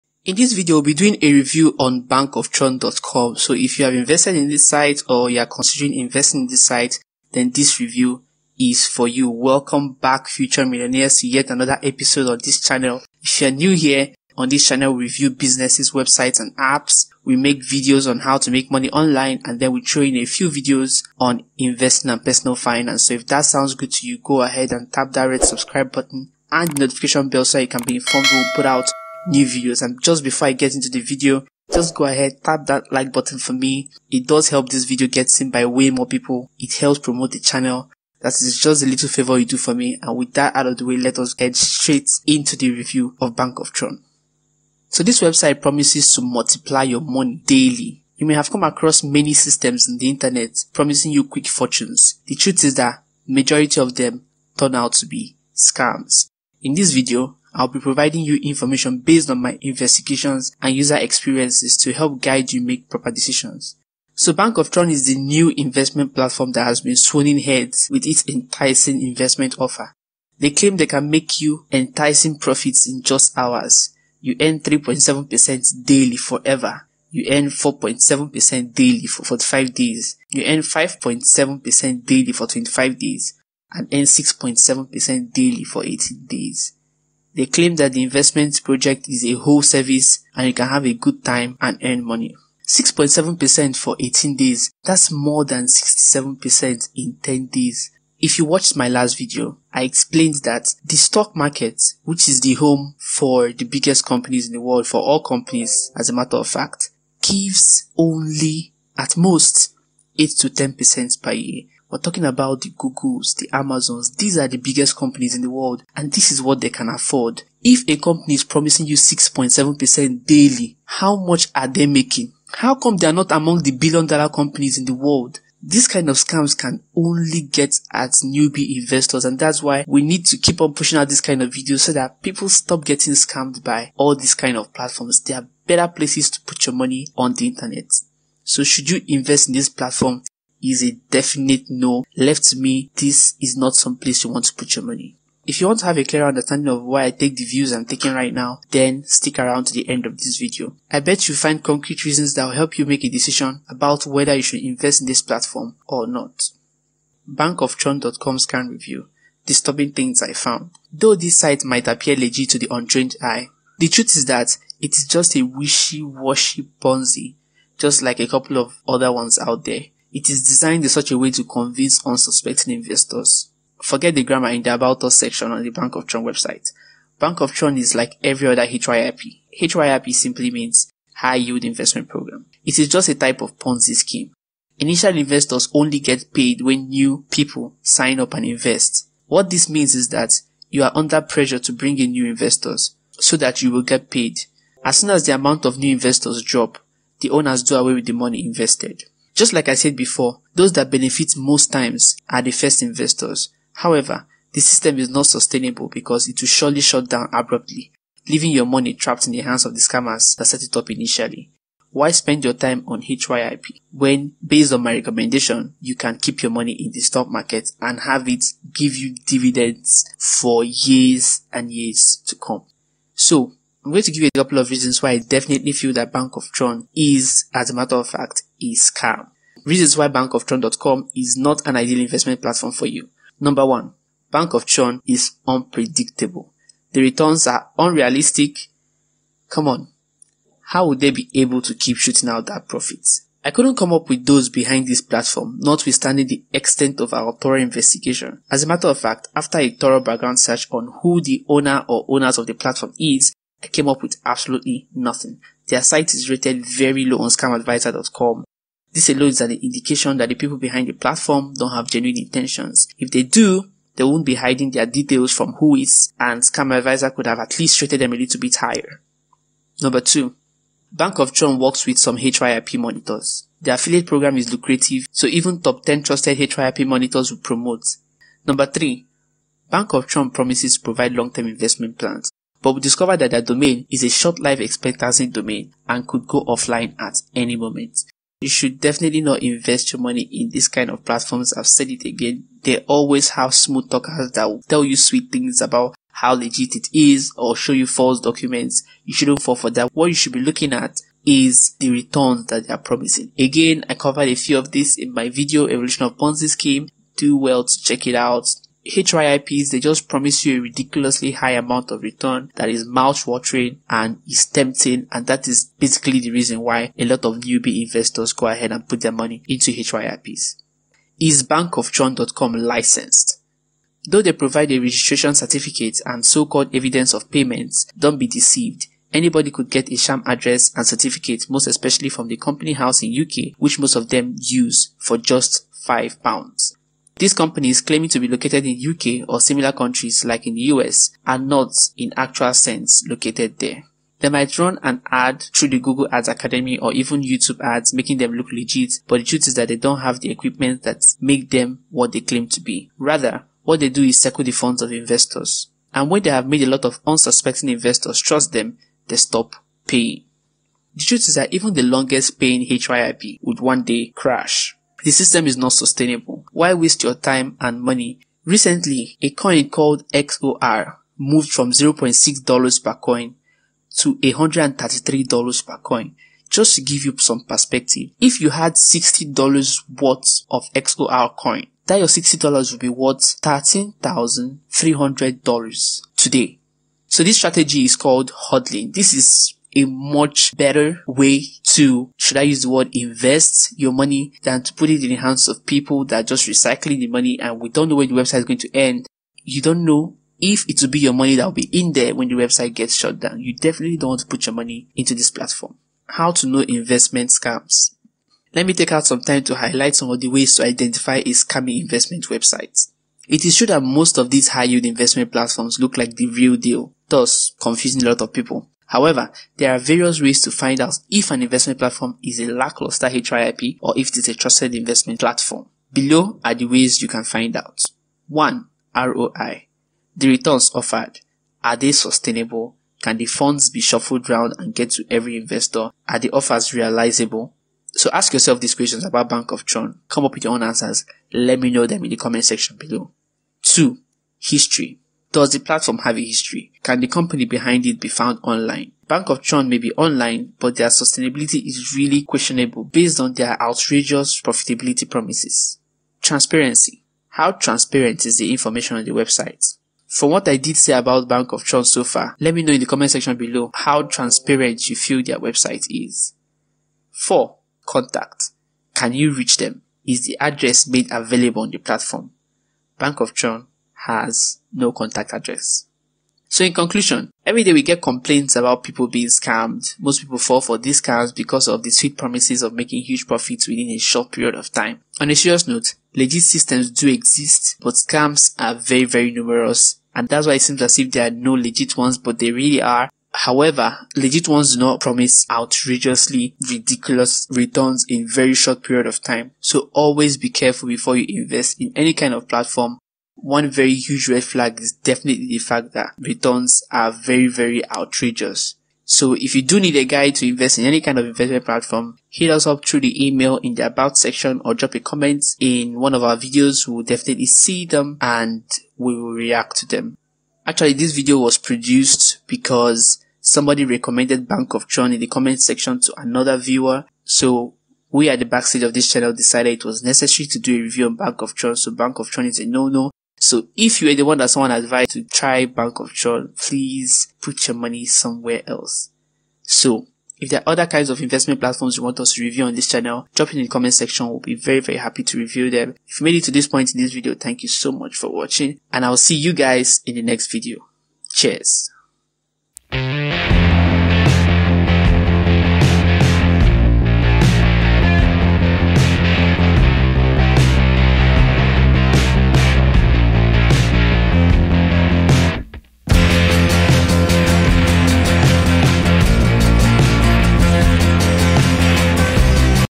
bank-of-tron.com Review | LEGIT or SCAM!!!
This website promises to multiply your money daily. Is bank-of-tron.com legit? You may have come across many systems on the internet promising you quick fortunes, the truth is that majority of them turn out to be scams.
In this review,  I  will provide you information based on my investigations and user experiences to help guide you make the proper decisions.
What Is Bank-of-tron.com?
Bank Of Tron is the new investment platform that has been swooning heads with its enticing investment offer. They claim they can make you enticing profits in just hours. You earn 3.7% daily forever, earn 4.7% daily for 45 days, earn 5.7% daily for 25 days, and earn 6.7% daily for 18 days. They claim that their investment project is a whole service where you can have a good time and earn money.